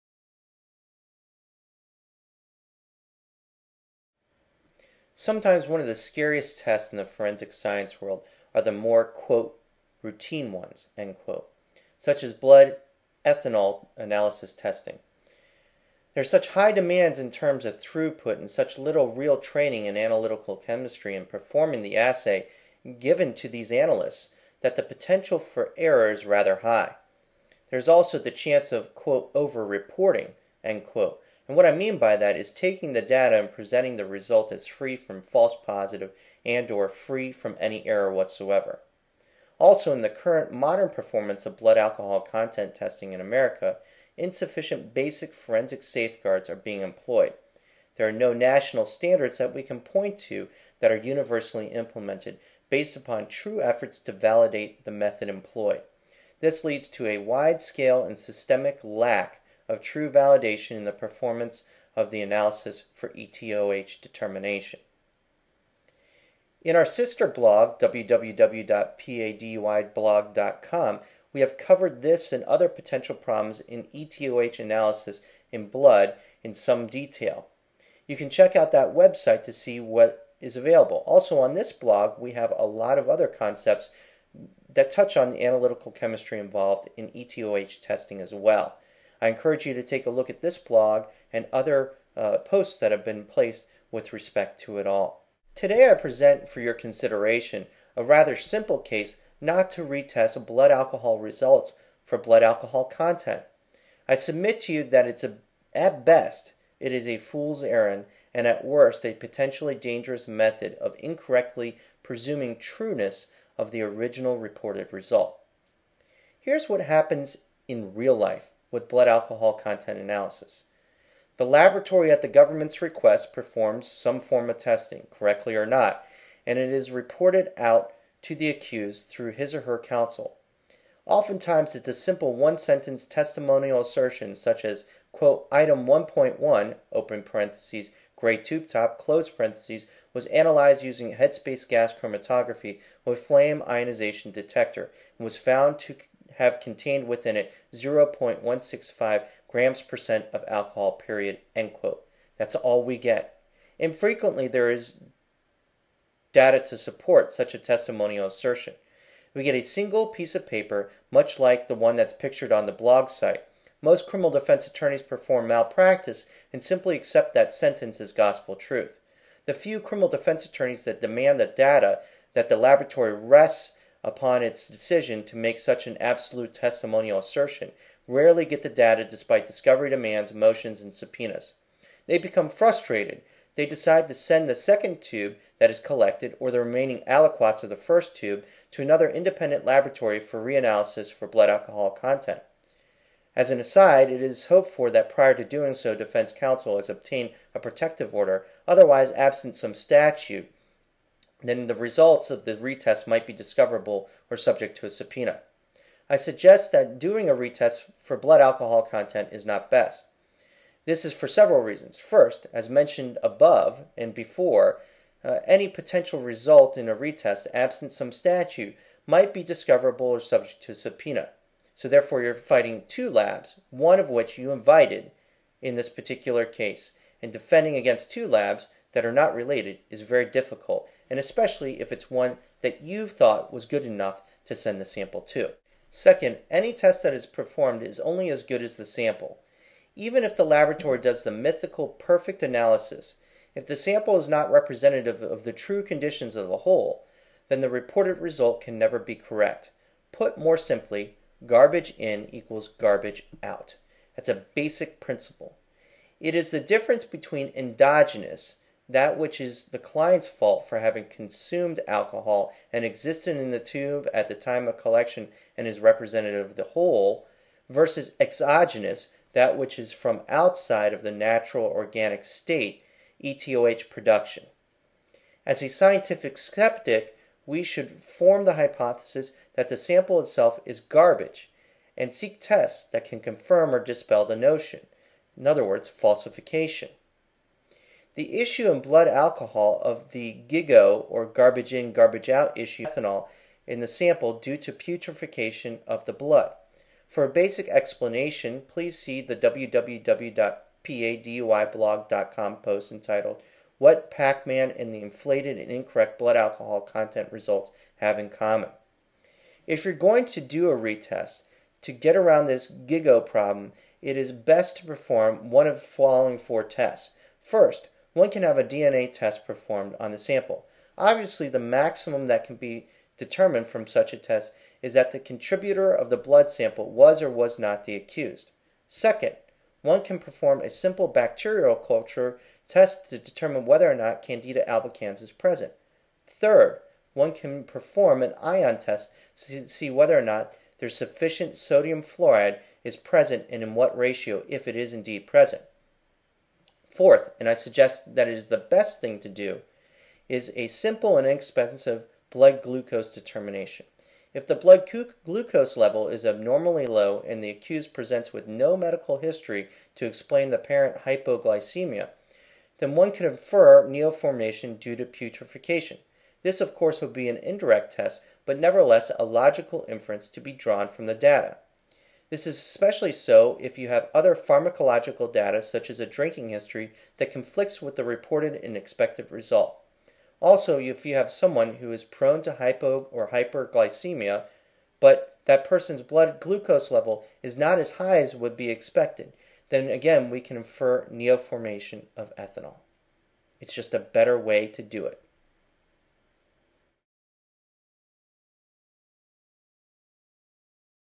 Retesting Blood Alcohol Results
Expert Pennsylvania forensic science attorney Justin J. McShane talks about Retesting Blood Alcohol Results.